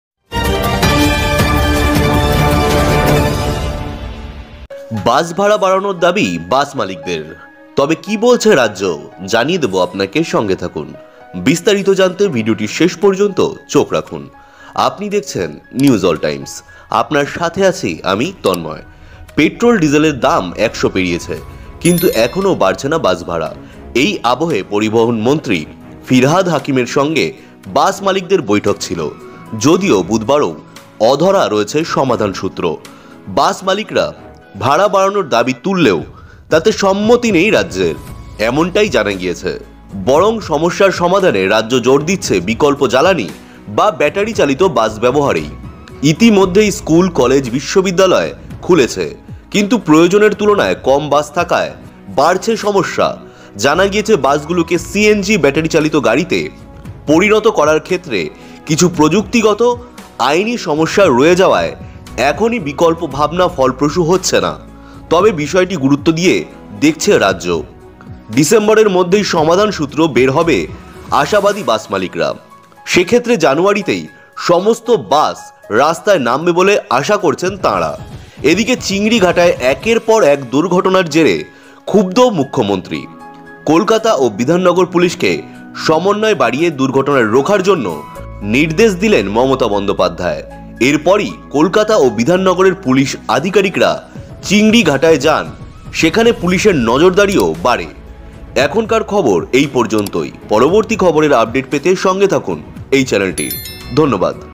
Bus owners demand increase in bus fares | বাস ভাড়া বৃদ্ধির দাবিতে বাস মালিকরা
Bus owners demand increase in bus fares | বাস ভাড়া বৃদ্ধির দাবিতে বাস মালিকরা |  @News All Times